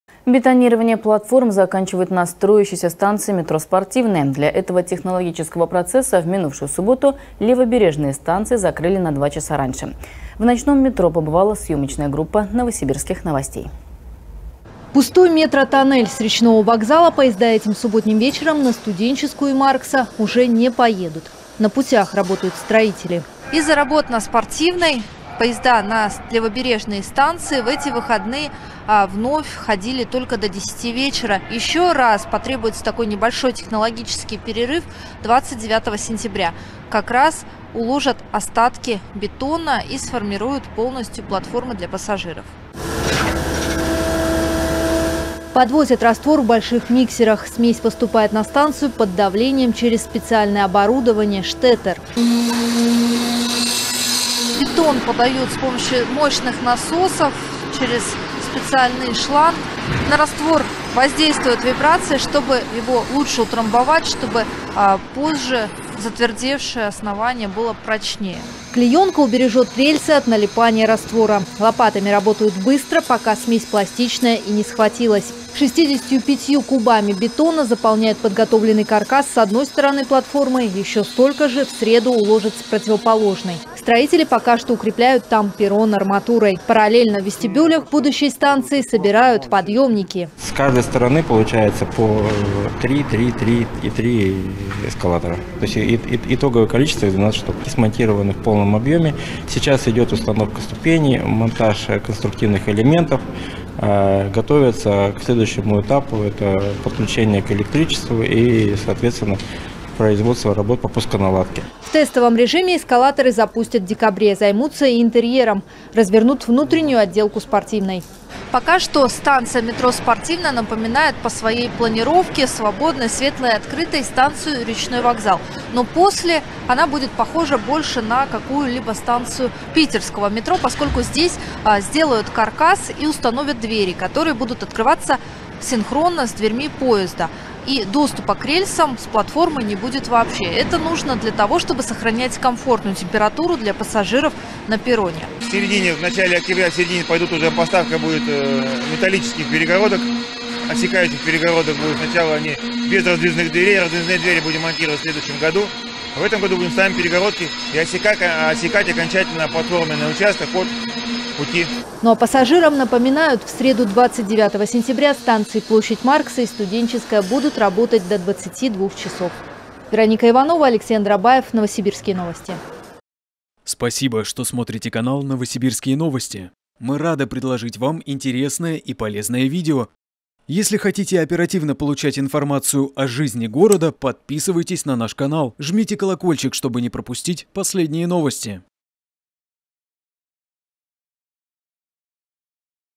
На станции метро «Спортивная» заканчивают бетонирование платформ
Для этого технологического процесса в минувшую субботу левобережные станции закрыли на два часа раньше. В ночном метро побывала съёмочная группа «Новосибирских новостей».